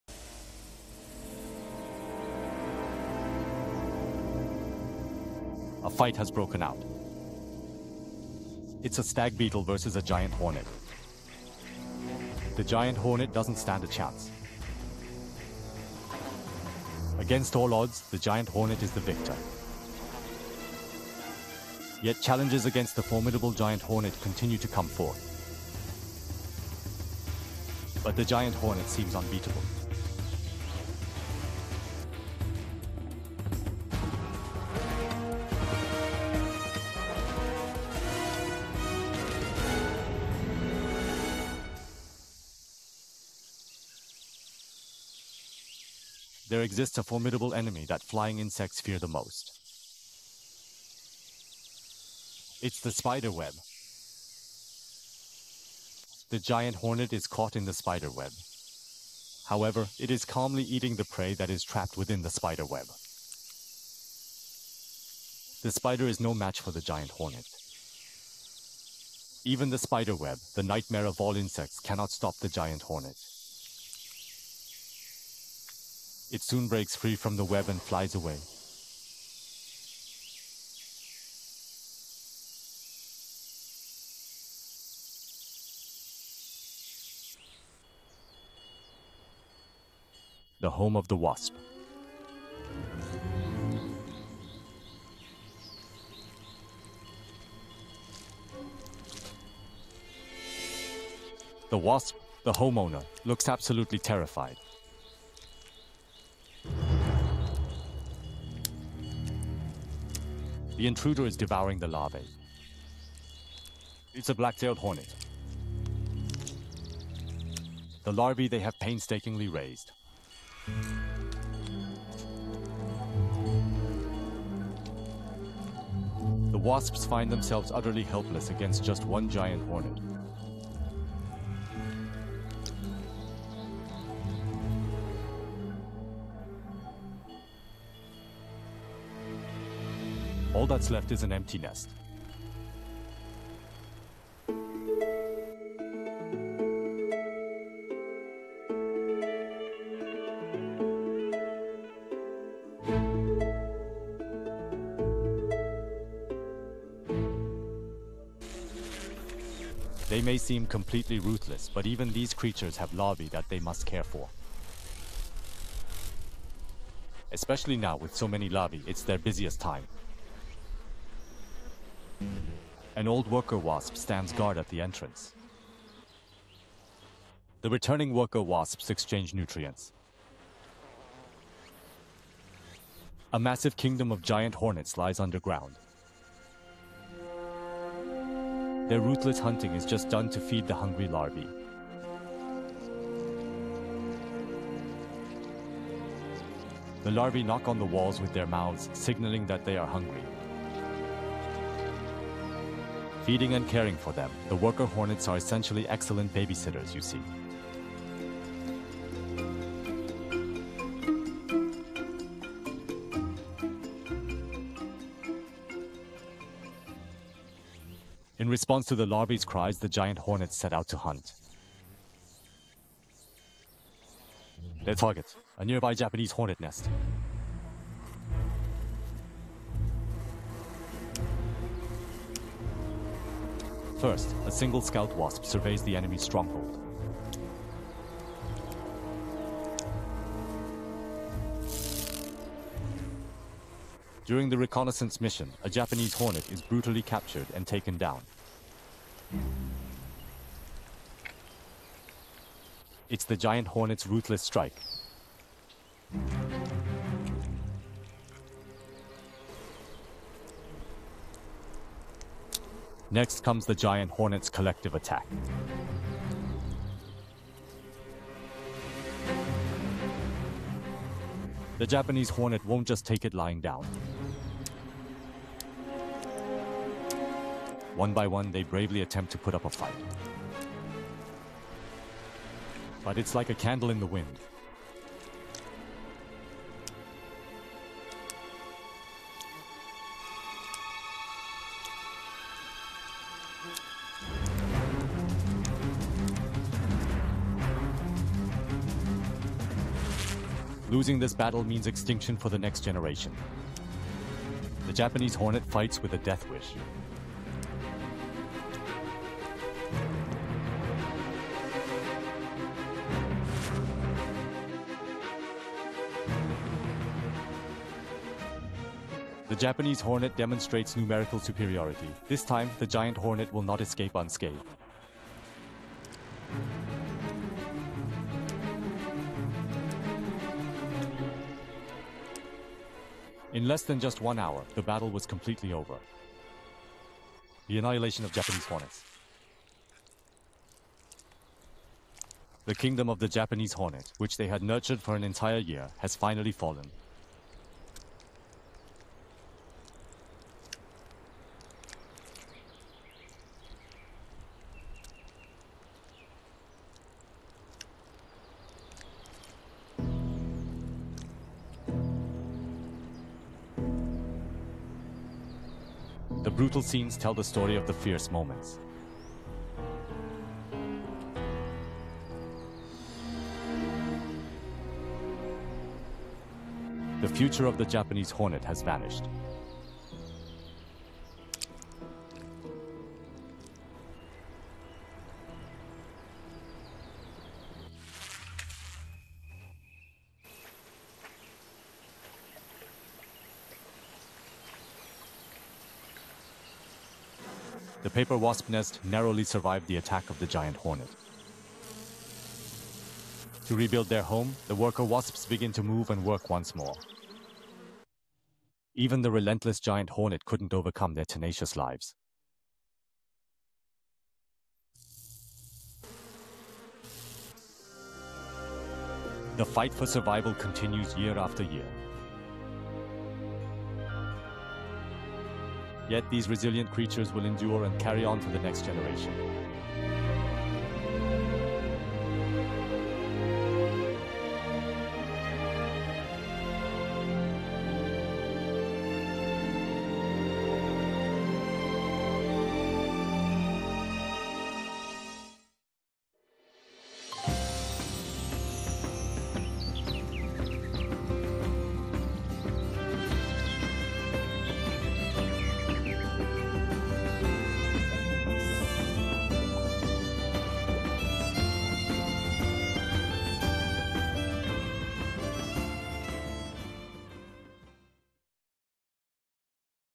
Nothing Can Stop This Insect: Inside the Giant Hornet’s Kill Zone
* Please note that this content has been edited using AI-powered dubbing technology.

The unchallenged outlaw of the insect world—Asian Giant Hornets.
From overpowering spiders and invading nests to wiping out entire hornet colonies, these fearsome predators reign with unmatched brutality.

Built to kill and driven to protect their young, these hornets embody both destruction and devotion.
This is nature’s most feared insect, caught in raw, relentless action.